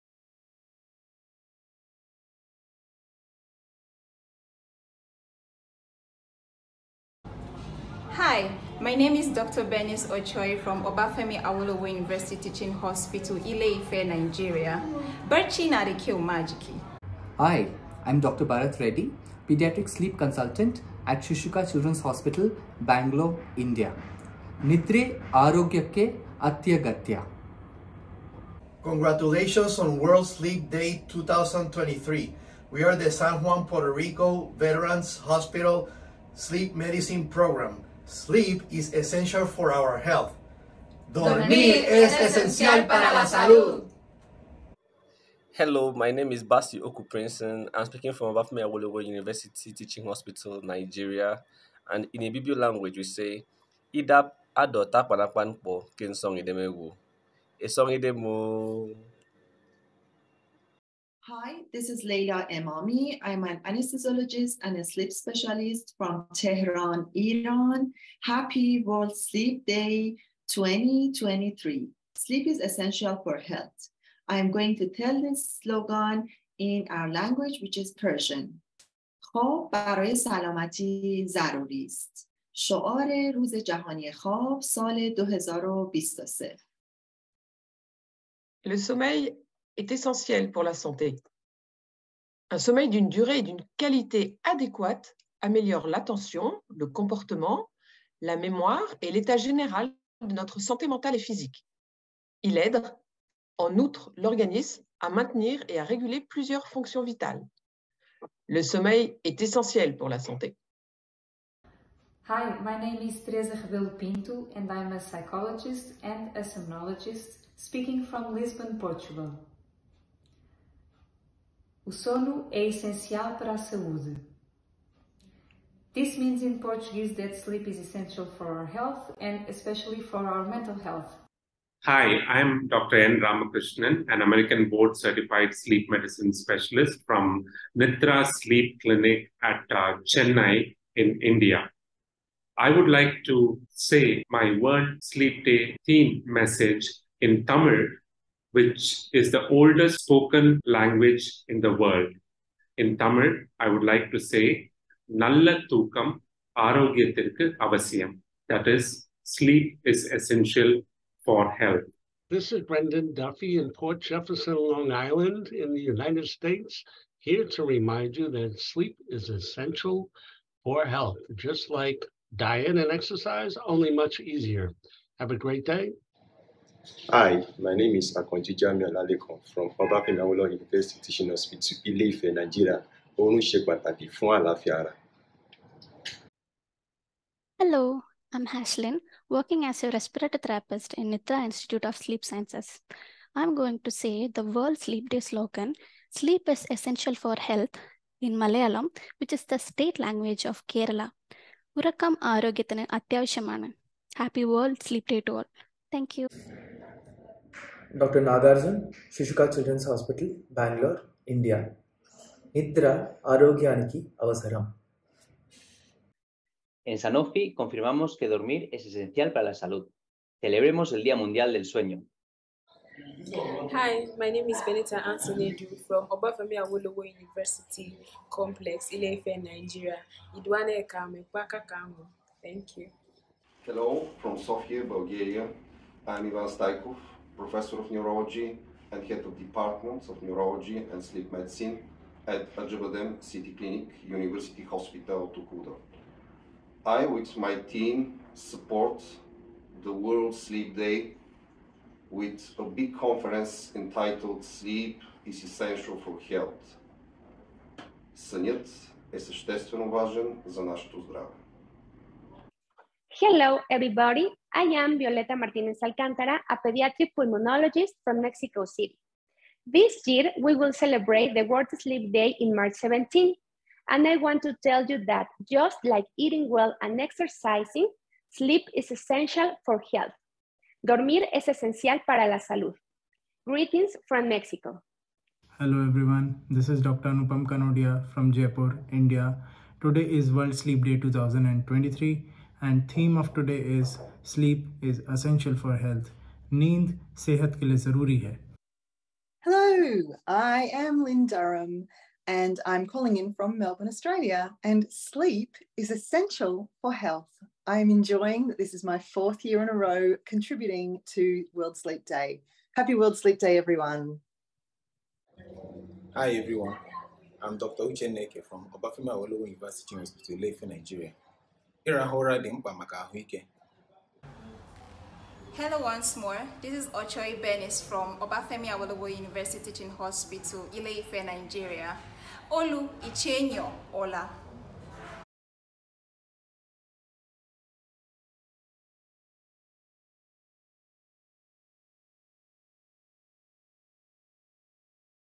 World Sleep Day 2023 Slogan Heard in 17 Languages!
Happy World Sleep Day 2023! This year's slogan is "Sleep is essential for health." Watch and listen to just some of our World Sleep Day Delegates say the slogan in their home languages -- 17 languages this time!

All around the world, sleep health advocates are organizing activities to raise awareness of sleep health in their communities. Sleep is essential to health, just like nutrition and exercise. Learn more about World Sleep Day and healthy sleep